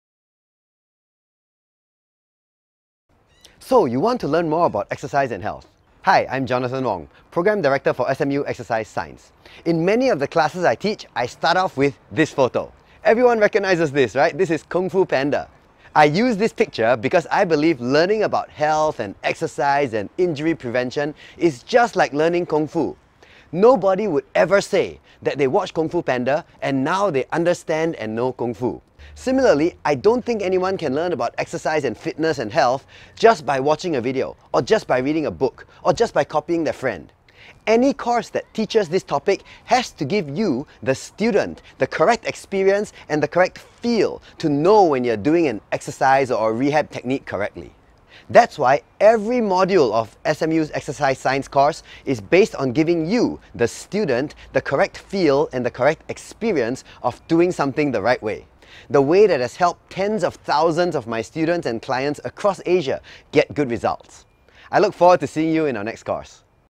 Introduction to Singapore Management University (SMU) Certificate of Exercise Science courses
Here is an intro to the exercise-related courses at Singapore Management University. Where I am the programme director for their exercise science programme.